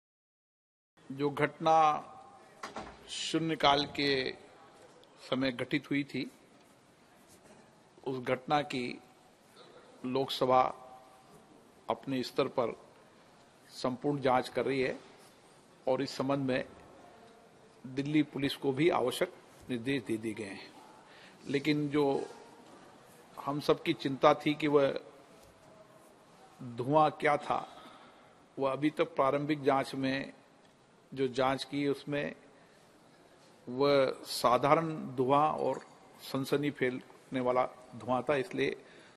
Lok Sabha Speaker Om Birla's Remarks on Security Breach In Lok Sabha
Lok Sabha Speaker Om Birla's Remarks on Security Breach In Lok Sabha लोकसभा में सुरक्षा उल्लंघन पर लोकसभा अध्यक्ष ओम बिरला की टिप्पणी